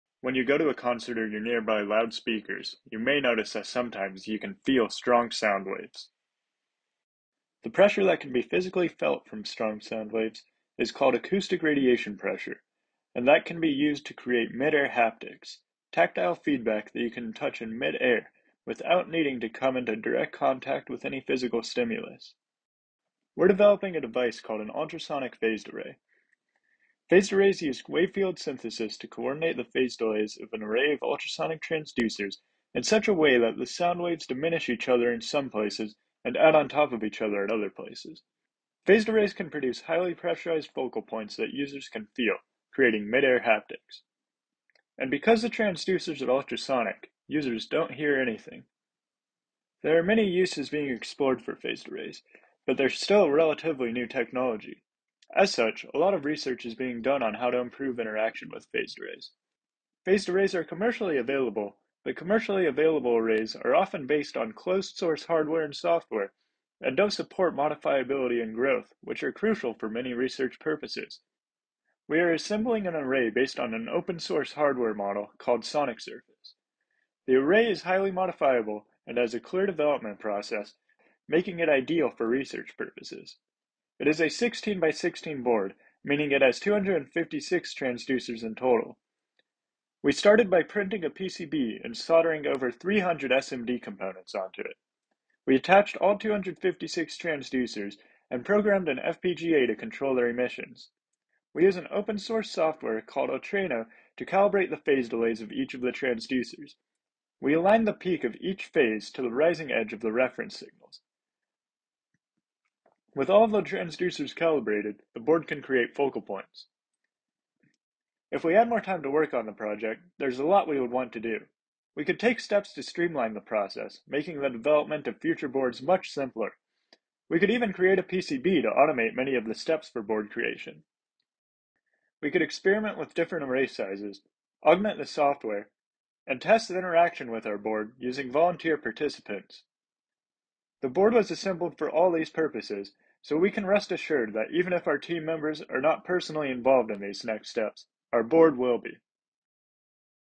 Ultrasonic Phased Array for Mid-Air Haptic Feedback - Capstone Presentation
This is a video demonstration of the capstone project titled "Ultrasonic Phased-Array for Mid-Air Haptics"
James Weber
James Fink
Ben Bloomgren
Blake Anderson